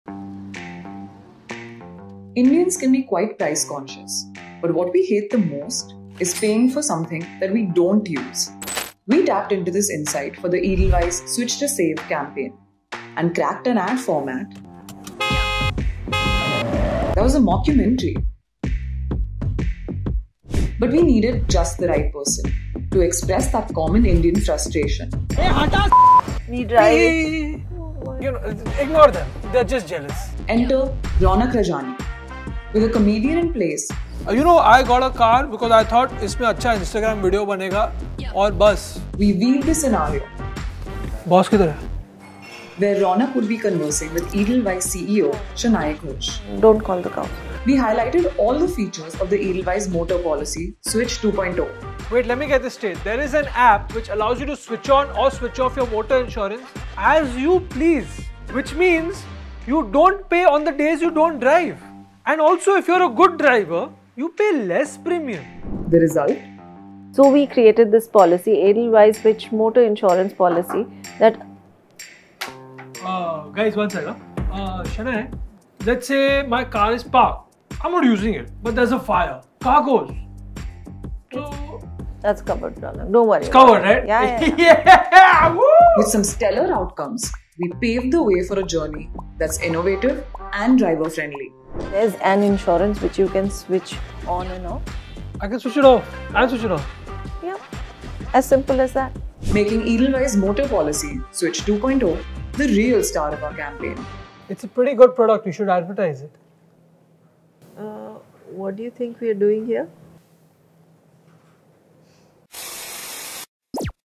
Edelweiss General Insurance : Switch to Save Case Study Video
To present a product this unique, we decided to move beyond a typical ad format. We needed something that effectively and entertainingly addressed the frustration Indians feel about paying for something they don’t use. So in a fresh mockumentary format, we had comedian Raunaq Rajani interacting with Edelweiss CEO Shanai Ghosh. With a humorous approach, our film narrative focussed on the key features of the Edelweiss Motor Policy – Switch 2.0.